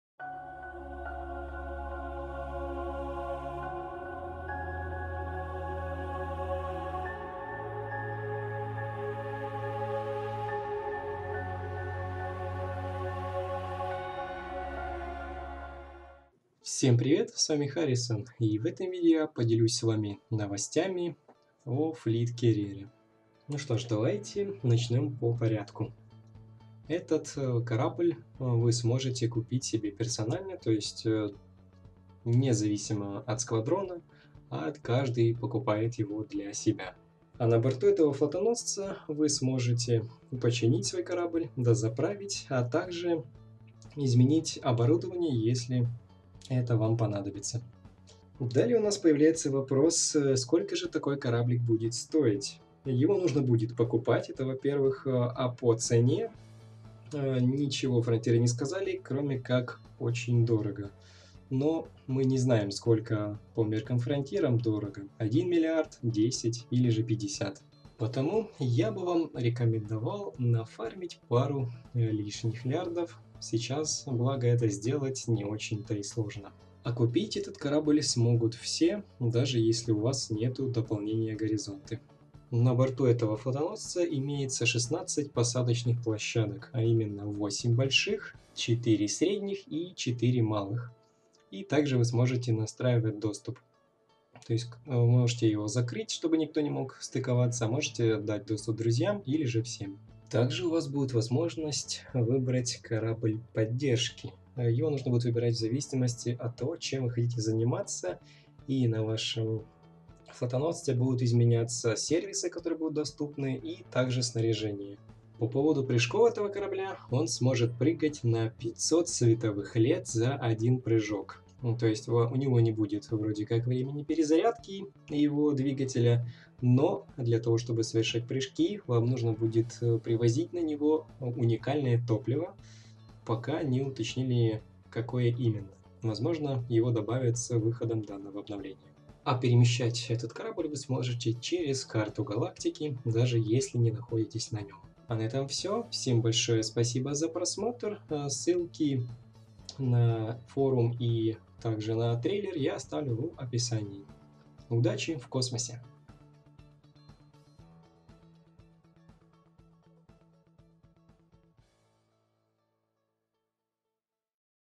Новости по Elite: Dangerous #2 | Fleet Carrier в декабре!
Новости о Fleet Carrier в Elite: Dangerous

Дата релиза - Декабрь 2019 года.

Удачи в космосе!